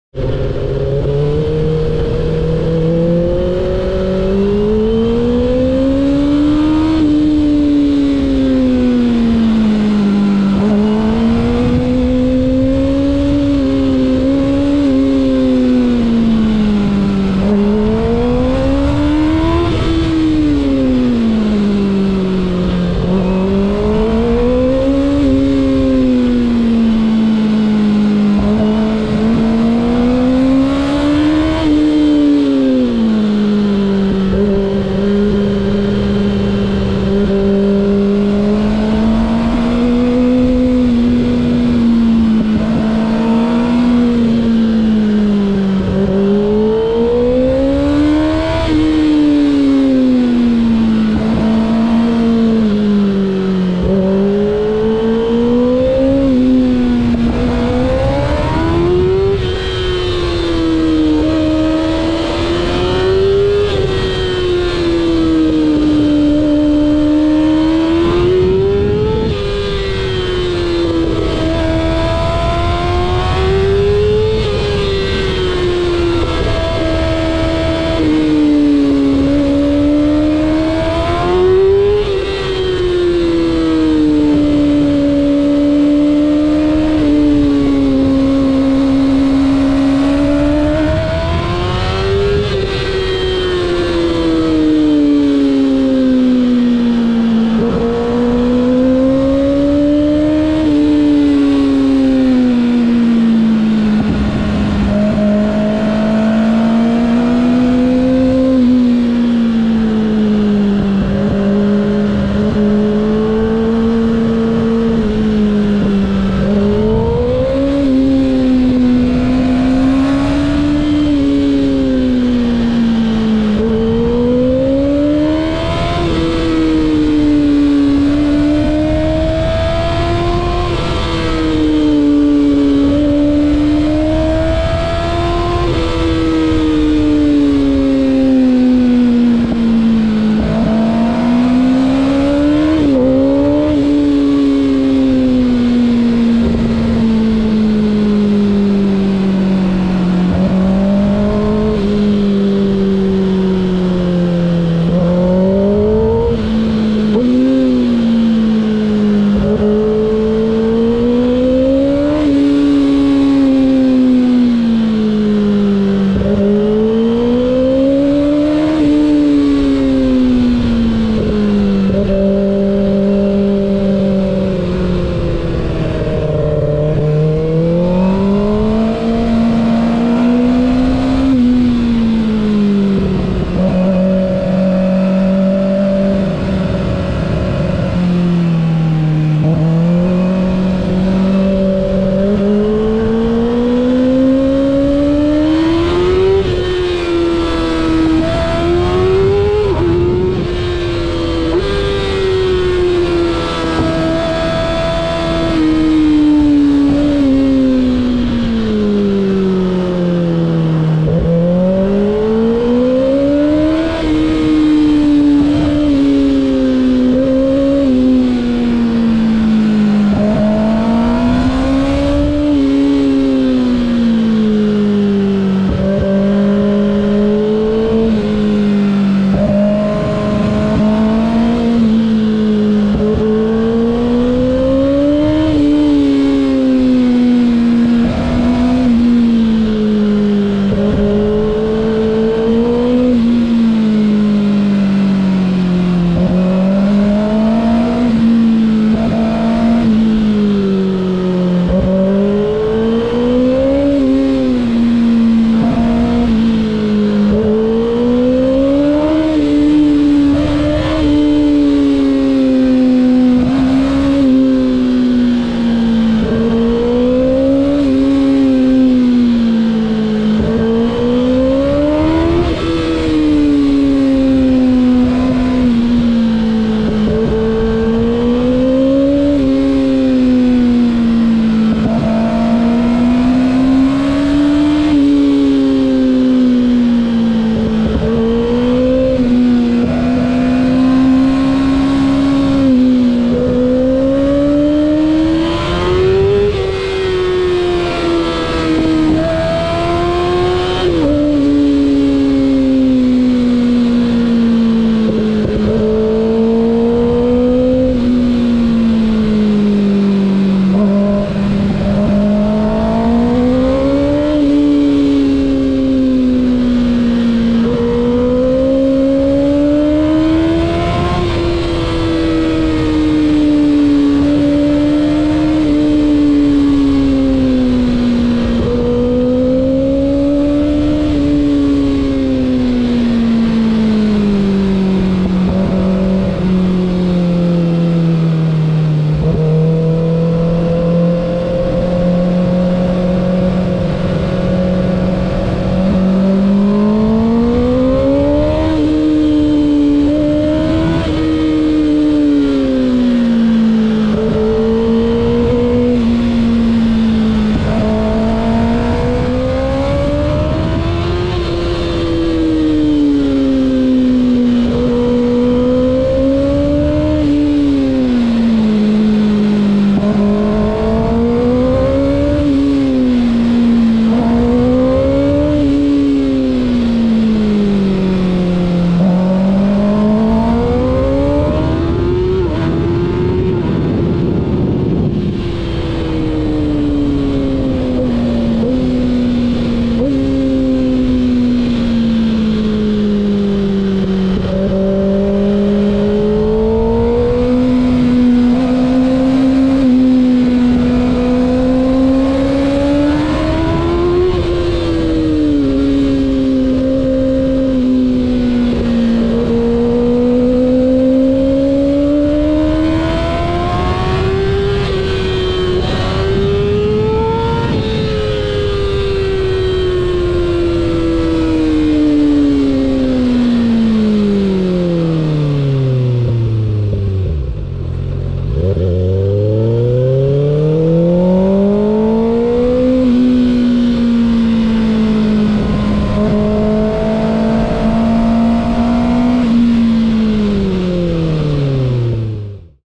2024 Ninja ZX6R Canyon Run Downhill
First day on the new 2024 Kawasaki Ninja and too it to one of the mountains near me for some quick runs in the twisties.